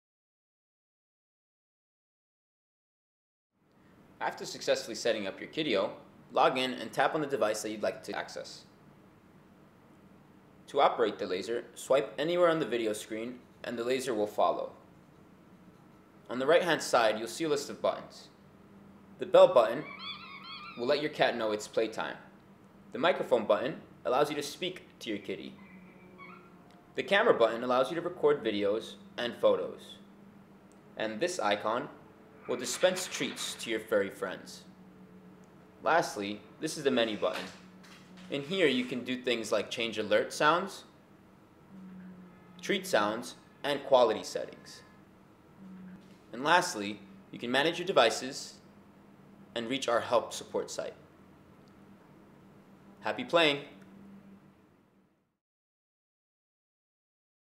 Kittyo: Basic App Use
This instructional video will show you the basic features of the Kittyo app.

The BELL button will let your cat know it’s playtime.
The MICROPHONE button allows you to speak to your Kitty.
The CAMERA button allows you to record videos and photos
The TREAT icon allows you to dispense treats to your furry friend.
The SETTINGS button allows you to change certain features such as alert and treat sounds.